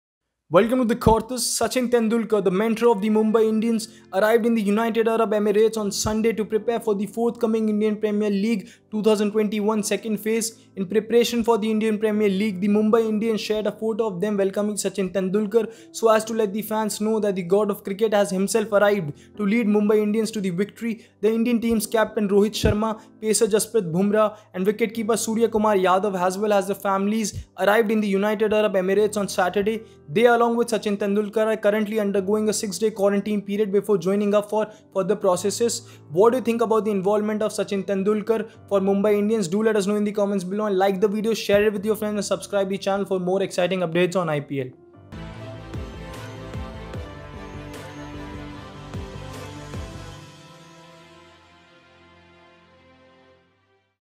Sachin Tendulkar To Mentor Mumbai Indians : Arrives In MI Before IPL
Sachin Tendulkar To Mentor Mumbai Indians : Sachin Tendulkar, the mentor of the Mumbai Indians, arrived in the United Arab Emirates on Sunday to prepare for the forthcoming Indian Premier League (IPL) 2021. Watch the video to know more...